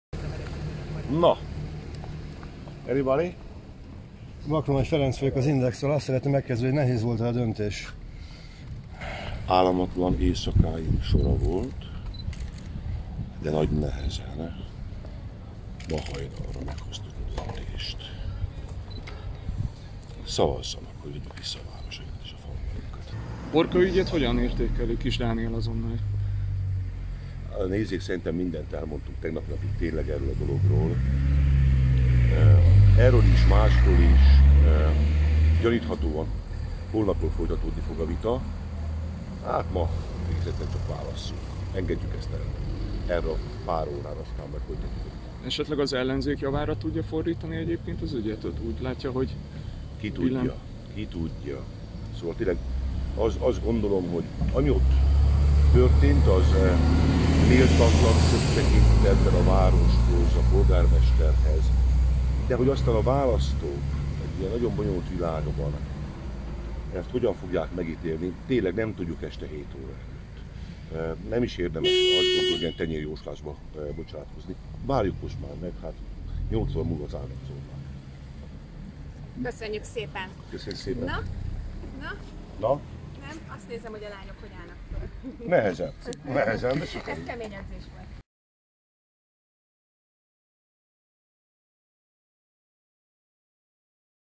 Gyurcsány: Hagyjuk ma a Borkai-ügyet! | AZONNALI
Megkérdeztük Gyurcsány Ferencet szavazás után, hogy szerinte előnyére válhat-e a Borkai-ügy az ellenzéknek. Videó!